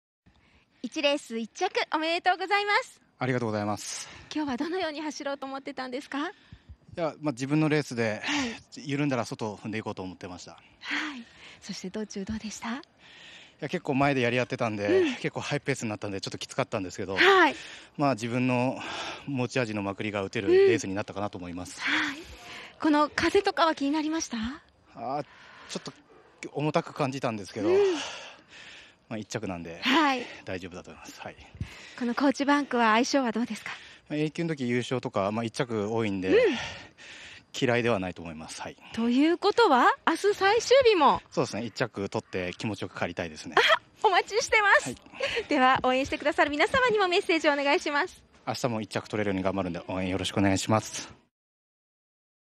2020/04/04 第1Ｒ ⑨小堺 浩二 勝利者インタビュー
高知市営 第１回 (ＧⅢ)   勝利者インタビュー 2020/04/04
開設７０周年記念よさこい賞争覇戦